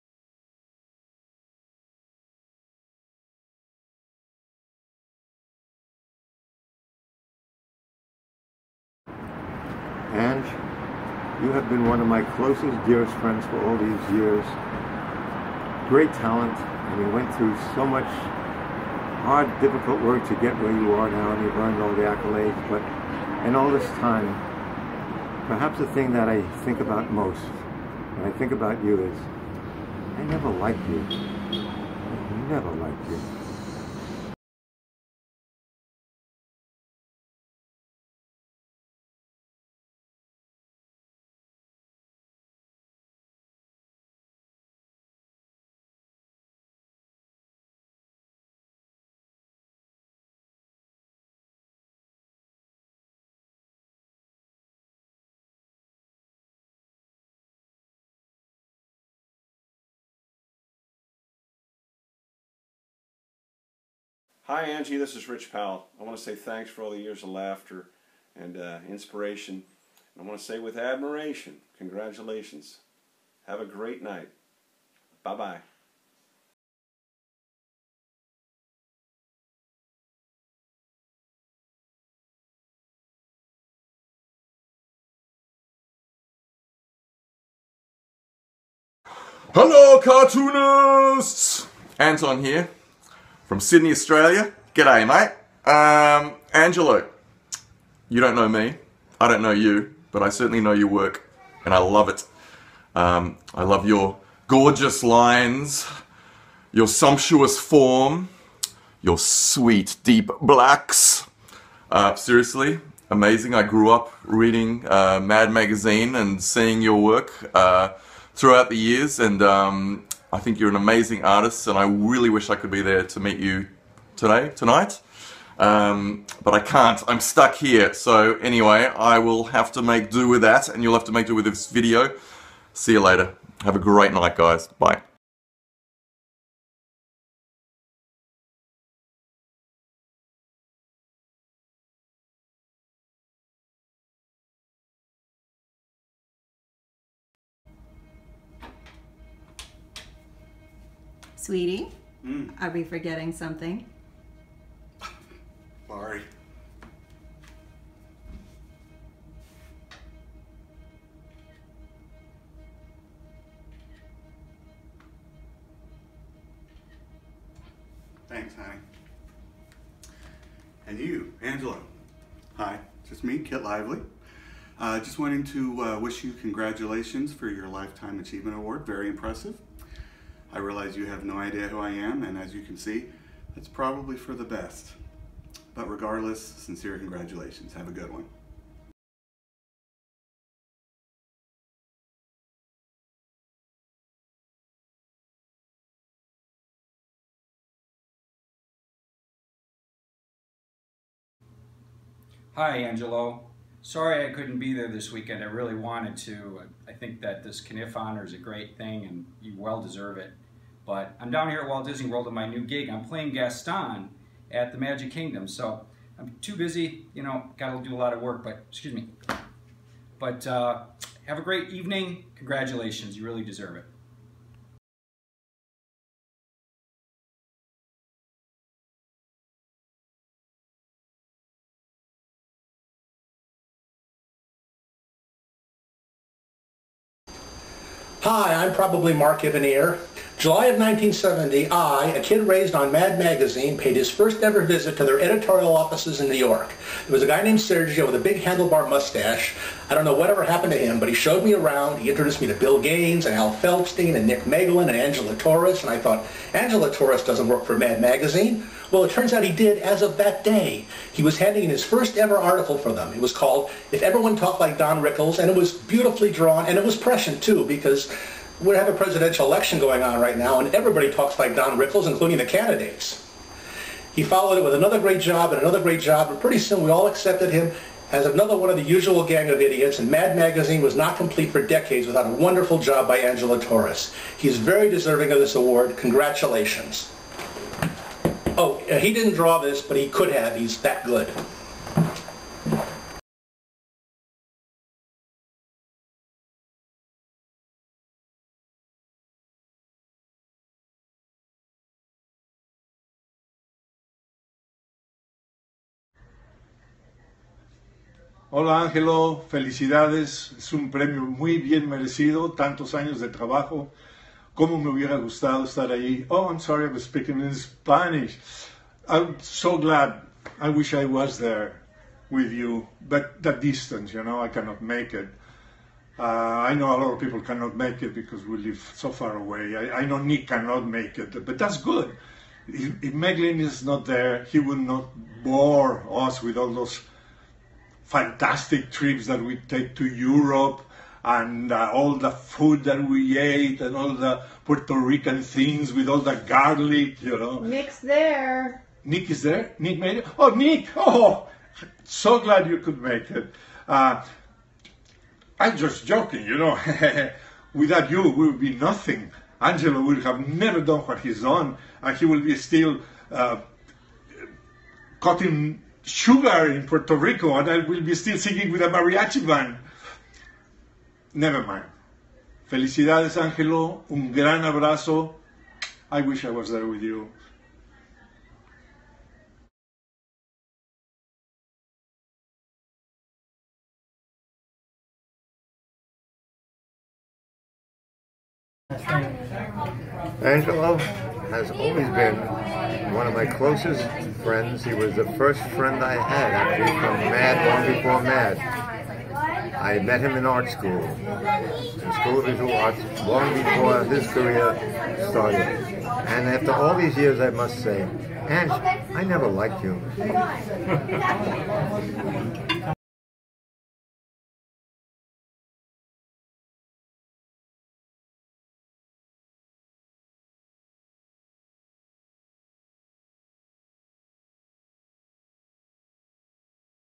Tribute video montage of artists giving props to Mad Magazine's Angelo Torres.
On October 24th, 2016, Angelo Torres was award the National Cartoonists Society's Milton Caniff Lifetime Achievement award, in New York City.

Some of his colleagues couldn't be there, so they sent in videos congratulating Ange.  Included are Sergio Aragones, Anton Emdin, Mark Evanier, Kit Lively, Nick Meglin, Rich Powell and Tom Richmond.

(FYI, No clue why the Nick Meglin segments are only audio.  If anyone can help me out, that'd be great.  Those bits are MP4, and this entire video was exported from Keynote.)